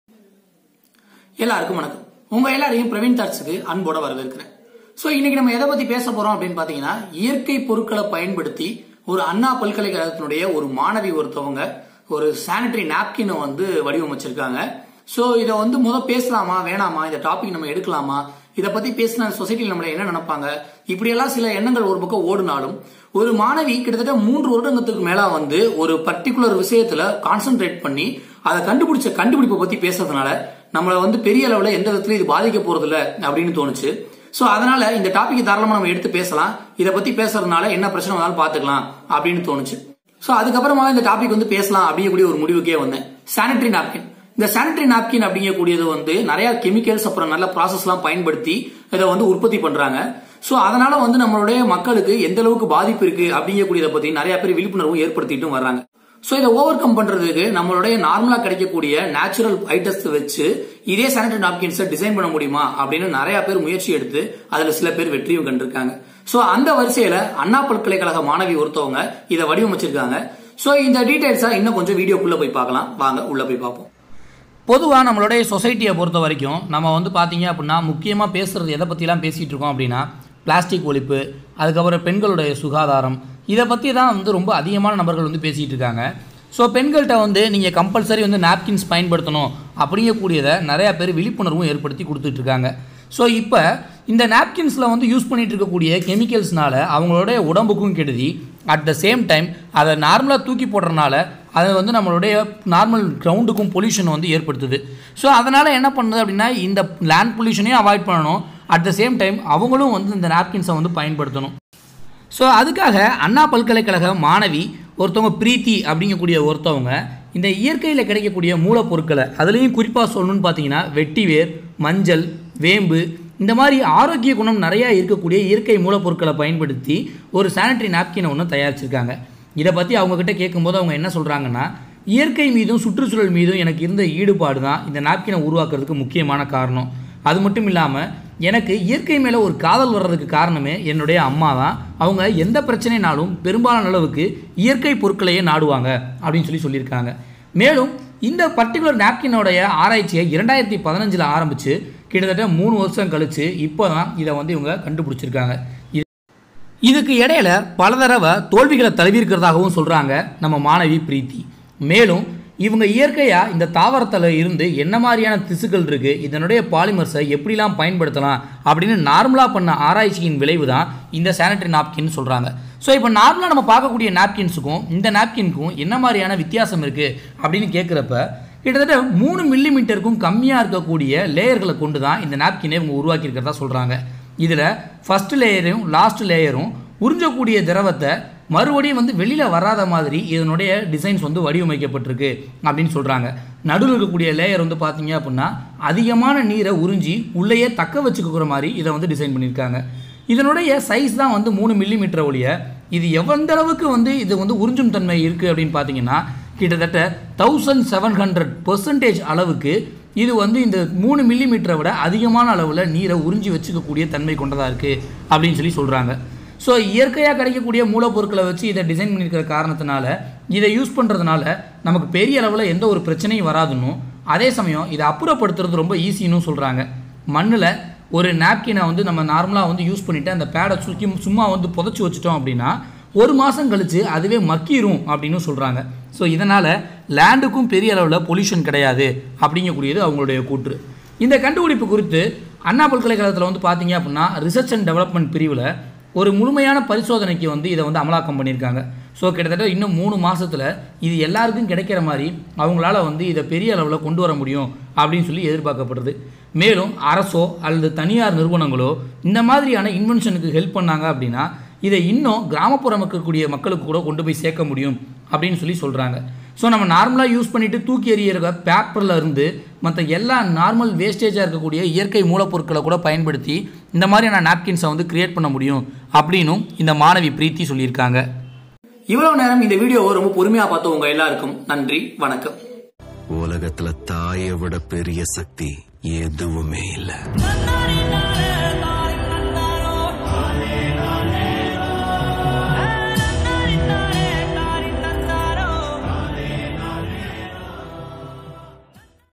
Natural Sanitary Napkin | New Invention of Anna University student | Praveen Thoughts
This video describes that the new invention of AnnaUniversity student which will be going to make a natural revolution for womens. I hope you people love this video and share this with your friends.